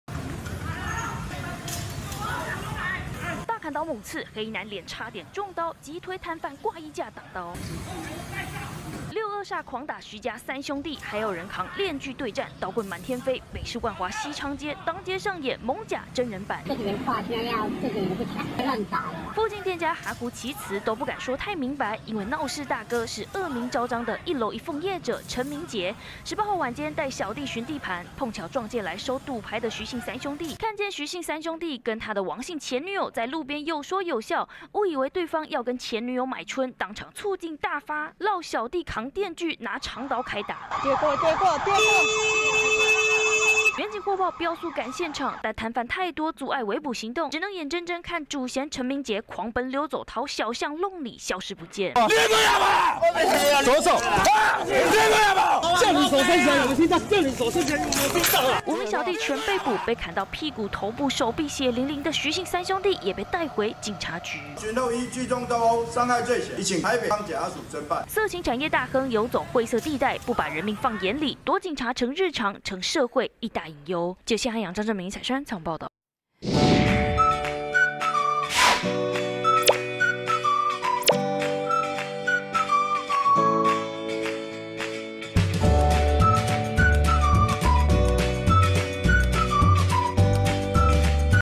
【每日必看】萬華角頭吃醋上演"艋舺" 撂小弟揮鏈鋸砍人 20240319
北市萬華西昌街夜市，昨天(19)晚間爆出鬥毆案，一度以為是攤商搶地盤衝突。警方到場發現竟然是，日前經營一樓一鳳落網的陳姓角頭看見王姓前女友，跟3名陌生男子交談，誤以為對方調戲前女友，醋勁大發，喊來5名小弟，拿鏈鋸，長刀砍人。警方陸續逮人，但陳姓主嫌仍然在逃。

負責小編:凱西編